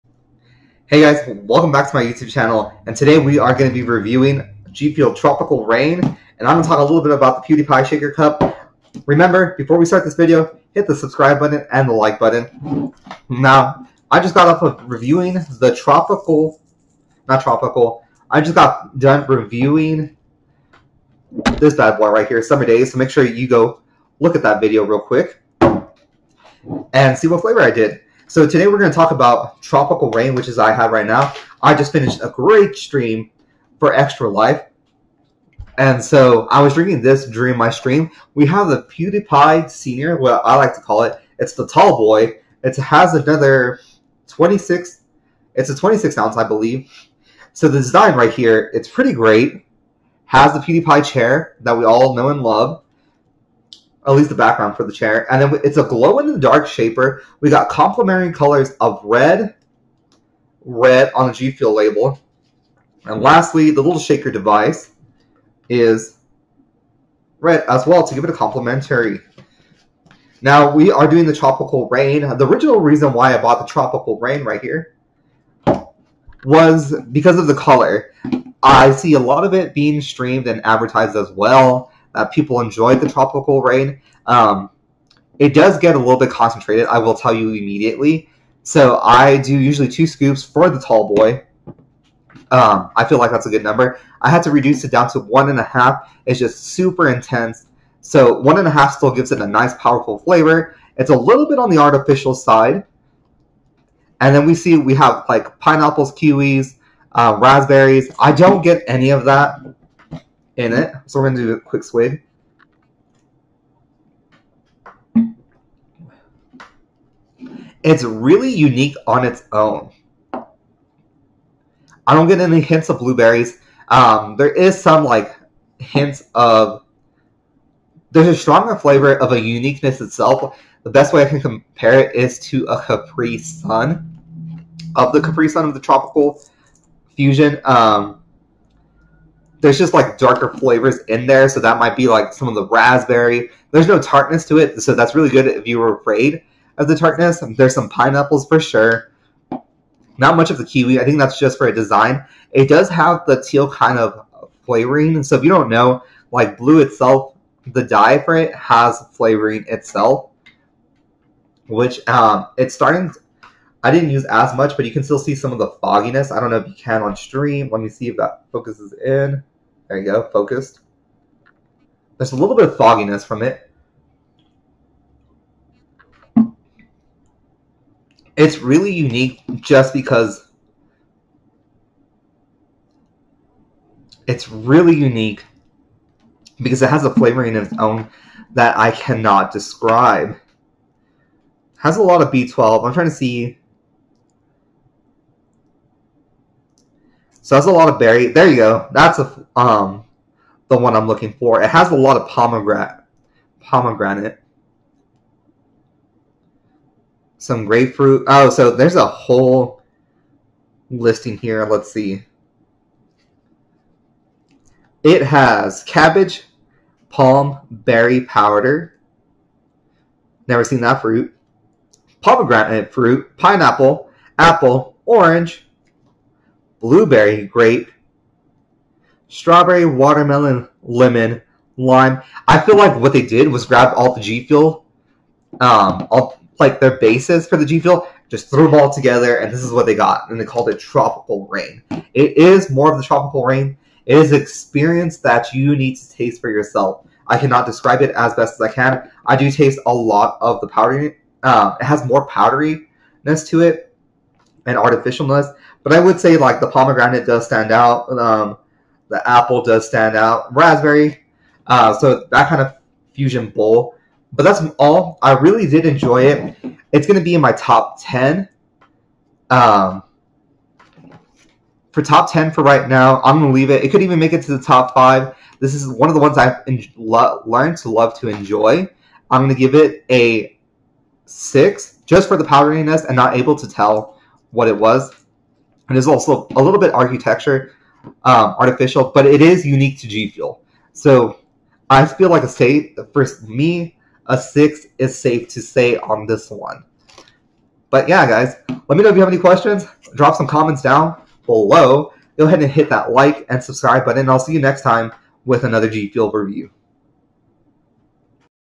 It's Raining up in here! Tropical Rain G Fuel Review!
The famous Tropical Rain flavor. I only bought it because I love the color. How does it taste though?